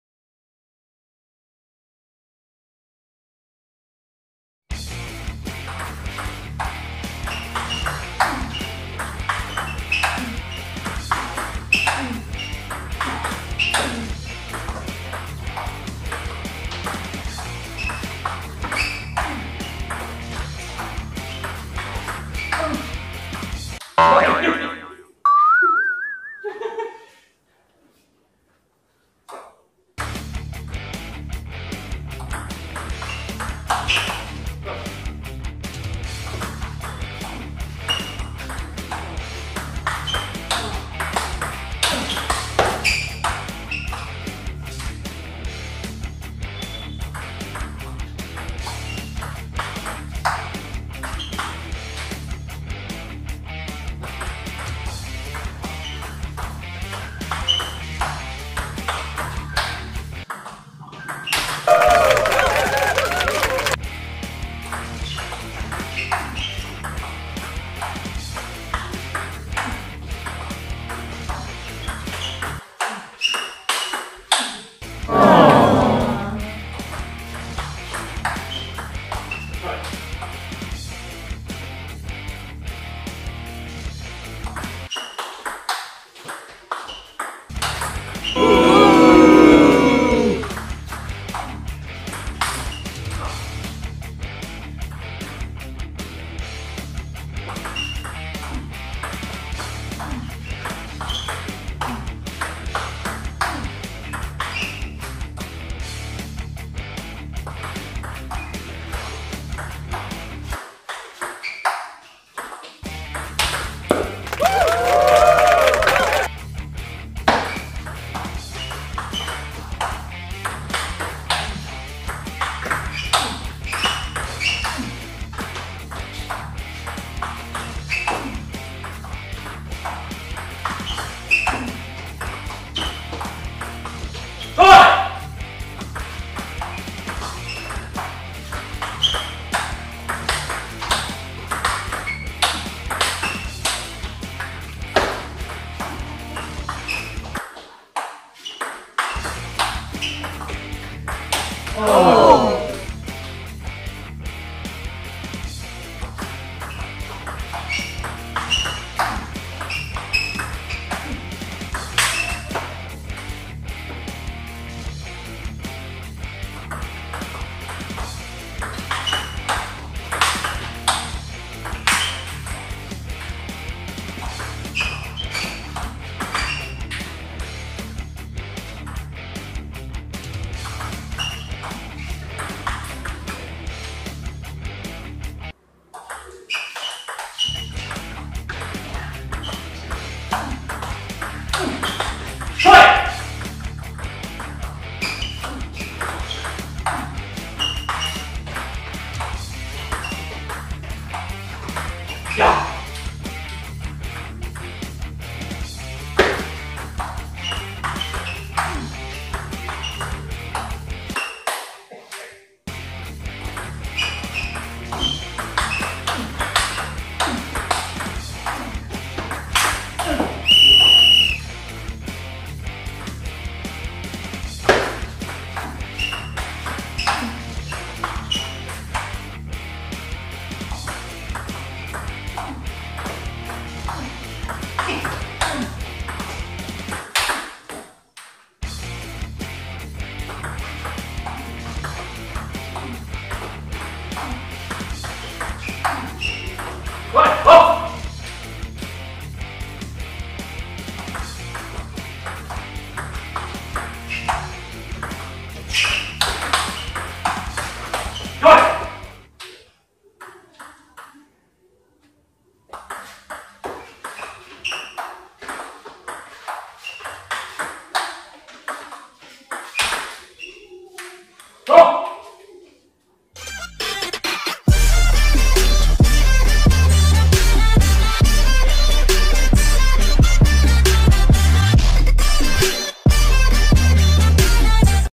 Table Tennis Attack vs Modern Defence (Match Highlights) 🏓👍☺️
few points from the match. enjoy . By the way i am using TSP SPECTOL on my Backhand and DONIC COPPA JO GOLD on my forehand. Short pips Backhand chopping is difficult to control but if u can master it, it's a great weapon for spin variation. heavy backspin to no spin balls.👍